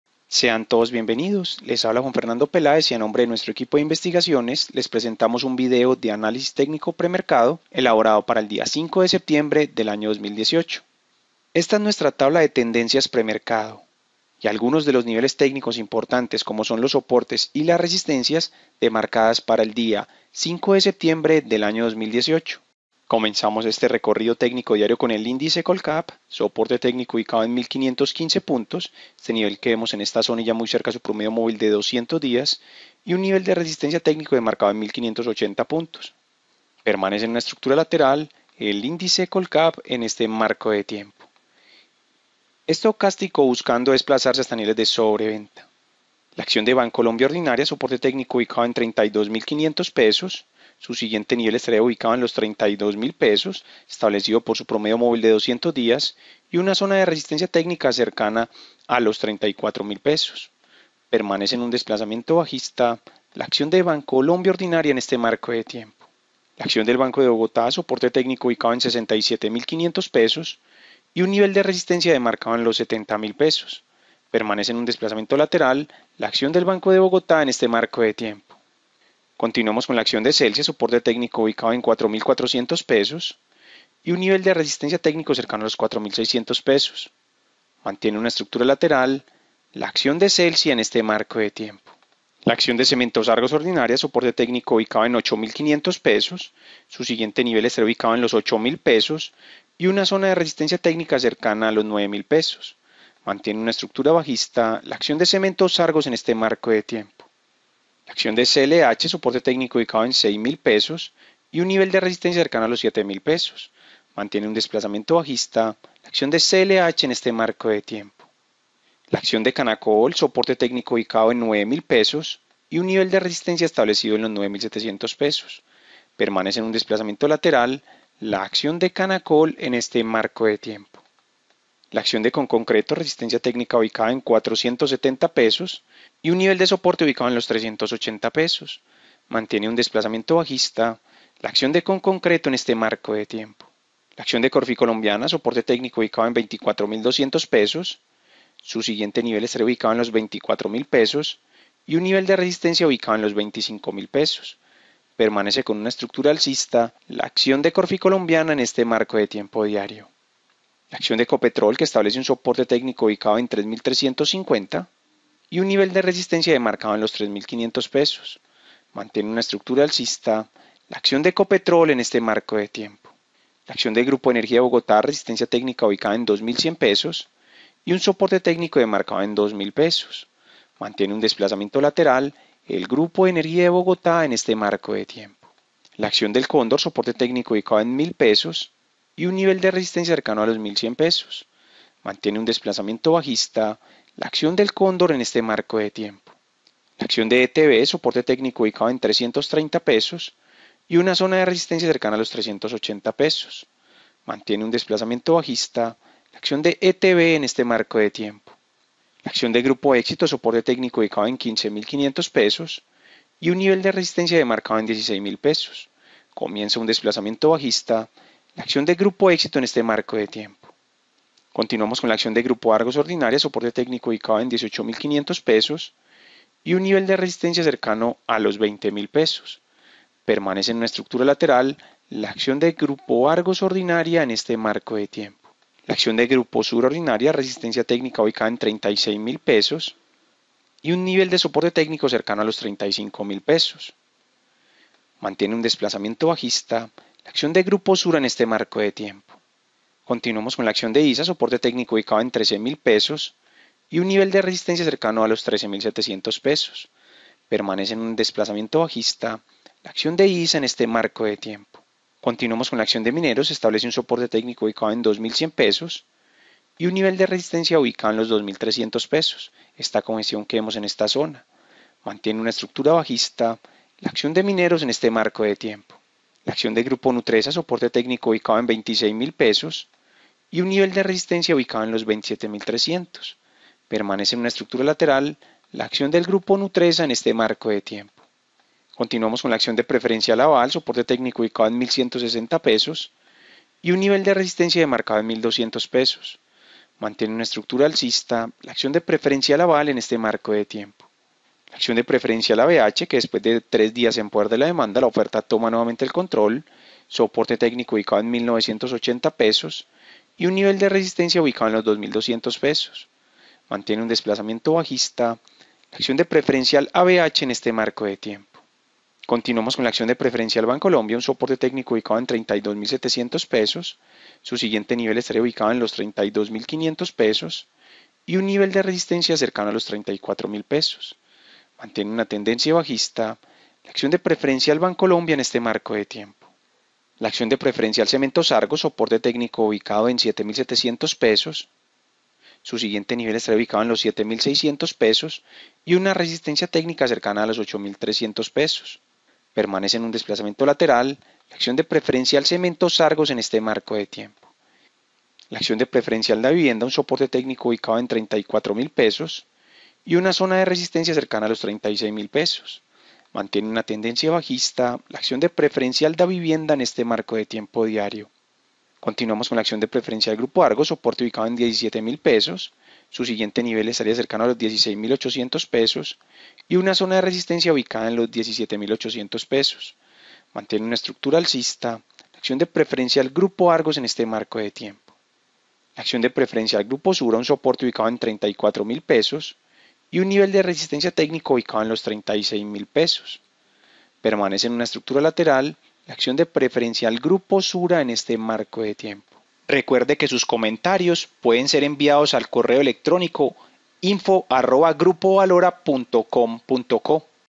Video análisis técnico - Septiembre 5 @BVCColombia COLCAP BVC ValoraAnalitik Suscríbase
El índice Colcap de la Bolsa de Valores de Colombia (BVC) mantiene zona de soporte de 1.515 puntos.

La acción de ETB se convierte en la más valorizada de la jornada.

Conconcreto continúa retrocediendo y se concierte en el mayor descenso del mercado.

Video de análisis técnico para entender el comportamiento de las acciones en el mercado bursátil colombiano, qué puede estar sucediendo con ellas y encontrar oportunidades de inversión.